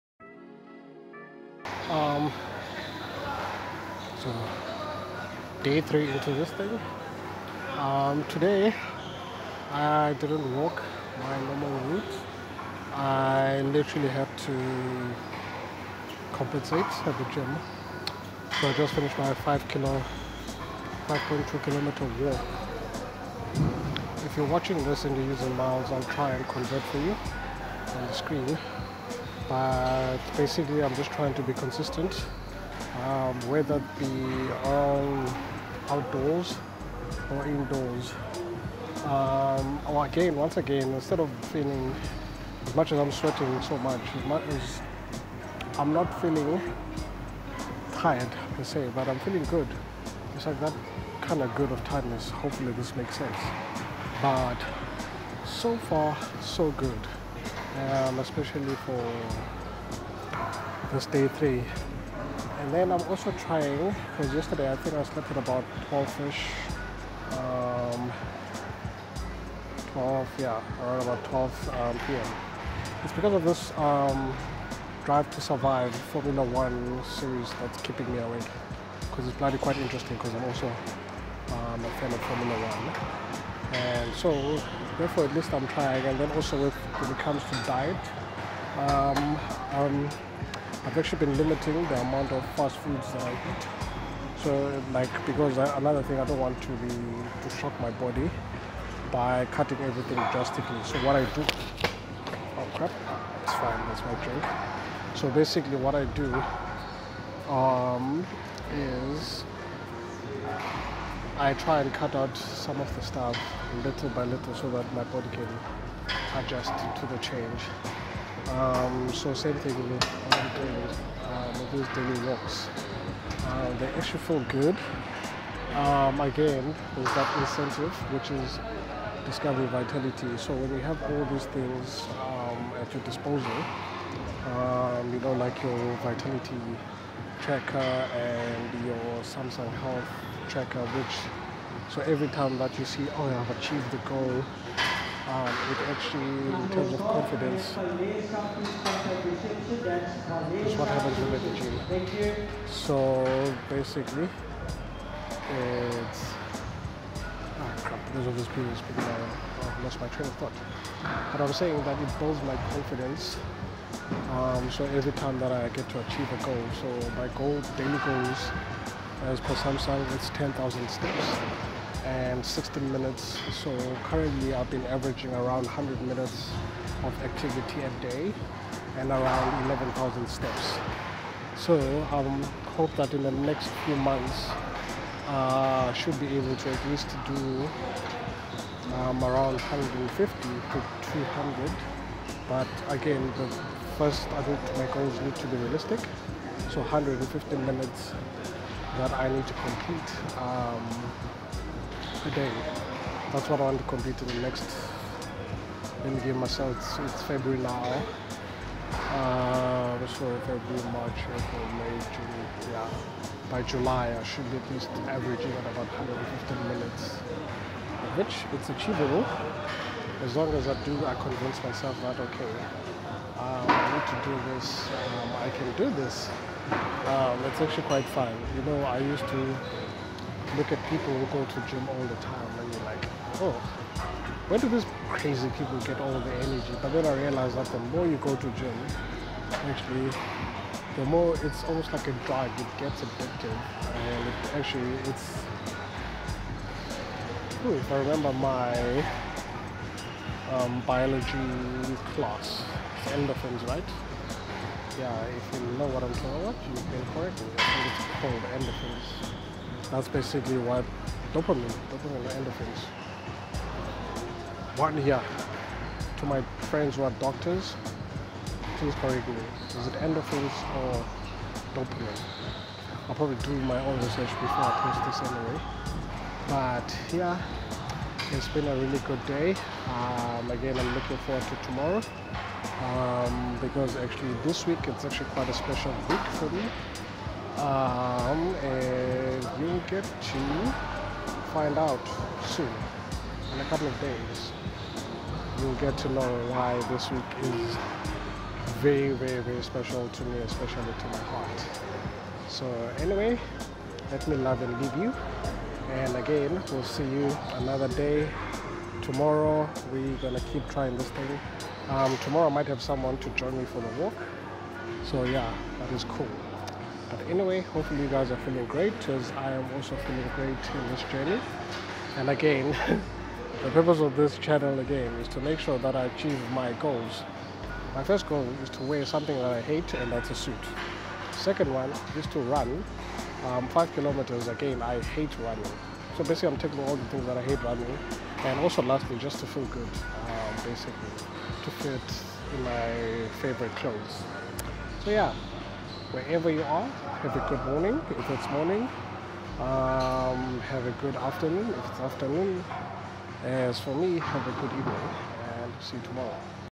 Day 3 - Going indoors
I have decided first and foremost to do these vlogs about my journey into taking daily walks and eventually transition into lots of running in a couple of months. These videos are my way of accountability to myself and hopefully entertaining to you. Please be advised all advice is welcome to assist with my journey.